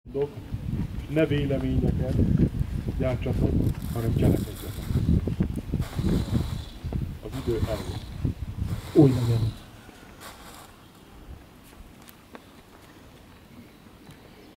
Nemzetségfői Üzenet
2014. évben 10. alkalommal került megrendezésre a Fényünnep Alsófarkasdon. Házigazda: Tóth Tibor és a Tudó Emberek Iskolája 30/971-6452. Közvetítő Daróczi Zoltán 30/9631-635. Szívükben és lelkükben is hagyományőrző embereket várunk a következő években is, hogy éljük is meg Őseink élő hagyományait. A Hun Törzsszövetség 24 Nemzetsége fogadja a látogatókat.